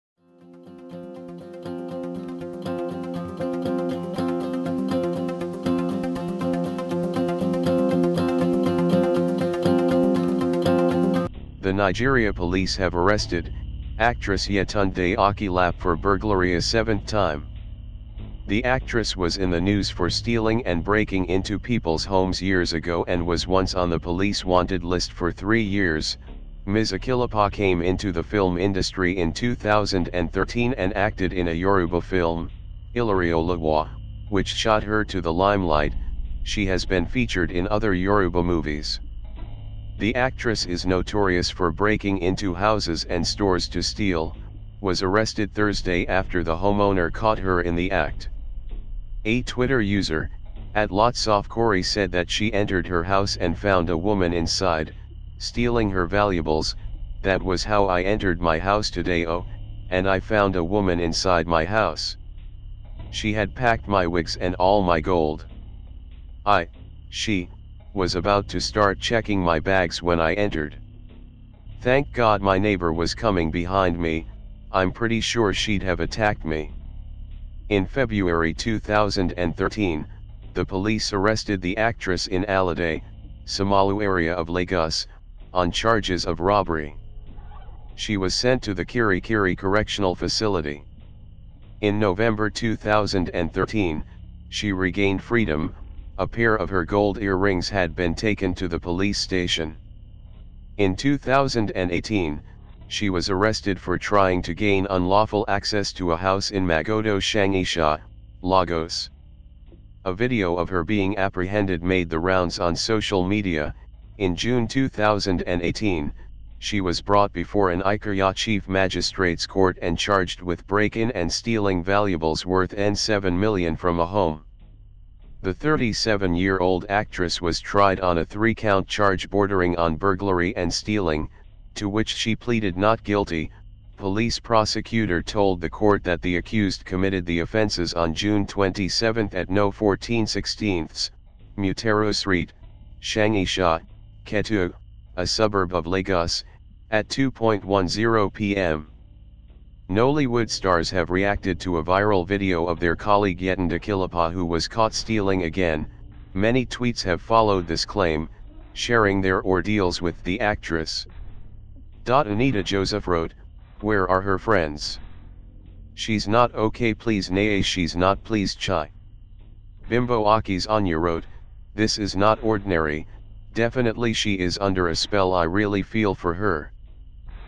Actress Yetunde Akilapa Caught Stealing again, This Isn’t Ordinary, She Is Under A Spell
The Nigeria Police have arrested, actress Yetunde Akilapa for burglary for the seventh time...
The actress was in the news for stealing and breaking into people’s homes years ago and was once on the police wanted list for three years…Ms. Akilapa came into the film industry in 2013 and acted in a Yoruba film, “Ileri-Oluwa’’, which shot her to the limelight…She has been featured in other Yoruba movies. The actress is notorious for breaking into houses and stores to steal, was arrested Thursday after the homeowner caught her in the act.